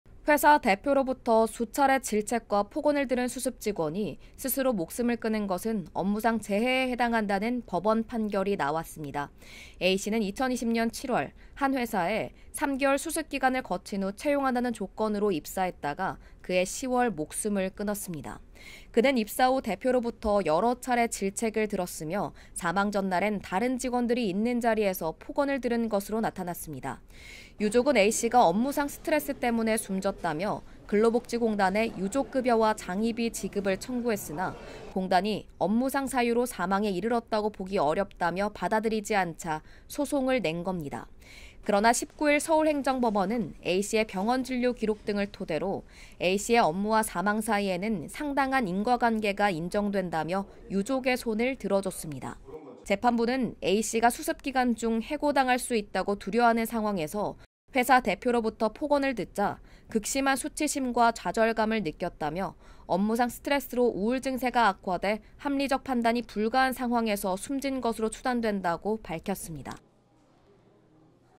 대표 폭언 들은 수습 직원, 결국... 법원 "업무상 재해 인정" / KNN
회사 대표로부터 수차례 질책과 폭언을 들은 수습직원이 스스로 목숨을 끊은 것은 업무상
재해에 해당한다는 법원 판결이 나왔습니다